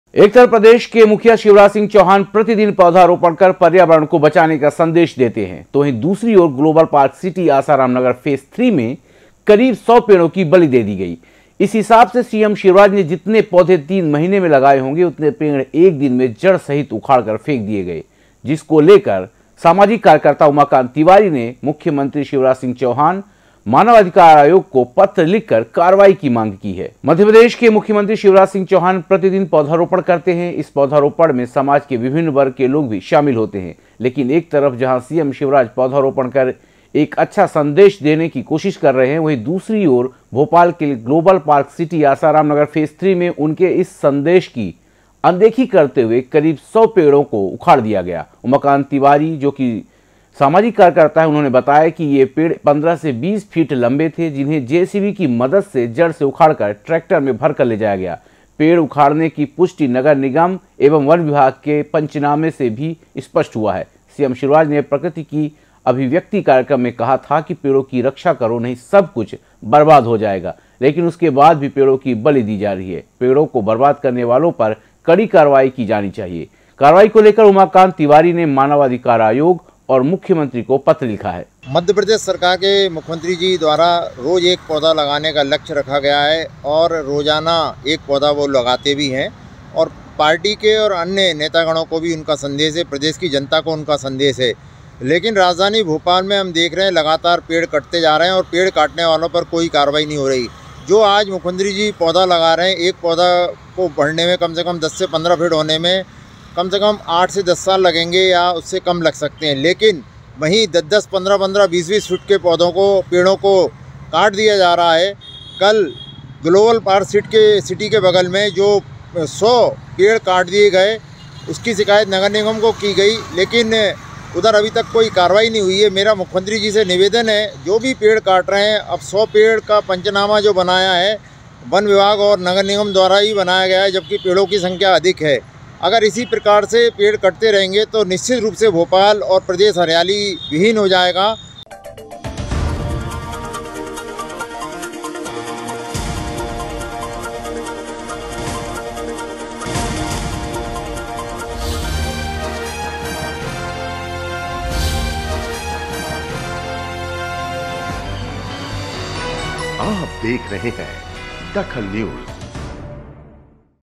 DAKHAL NEWS आसाराम नगर में करीब सौ पेड़ों की दी गई बलि
आसाराम नगर में करीब सौ पेड़ों की दी गई बलि
सीएम के पर्यावरण बचाने के संदेश की अनदेखी
मानव अधिकार आयोग ,मुख्यमंत्री से कार्रवाई की मांग